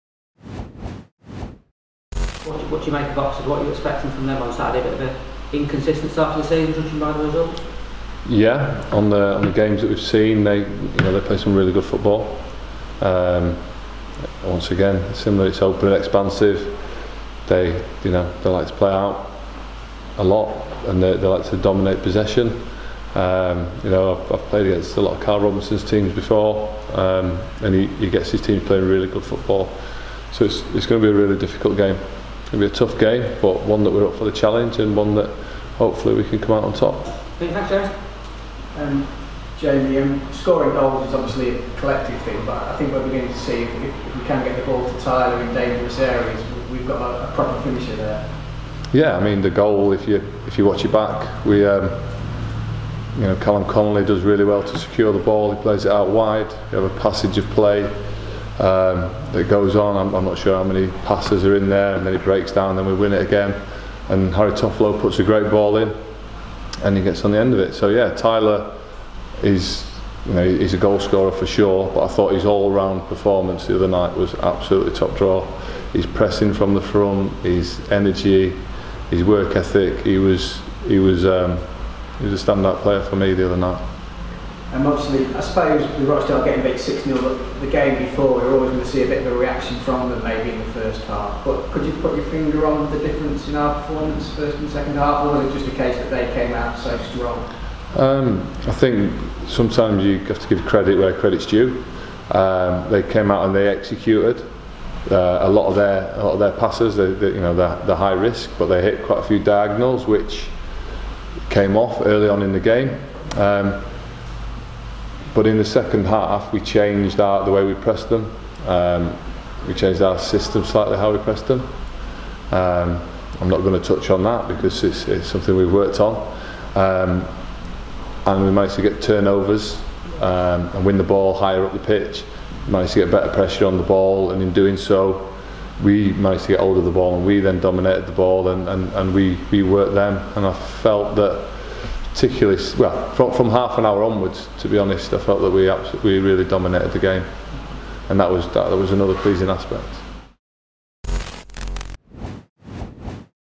🎥 Interview | Jamie McCombe Pre Oxford United
Jamie McCombe previews the Sky Bet League One match against Oxford United.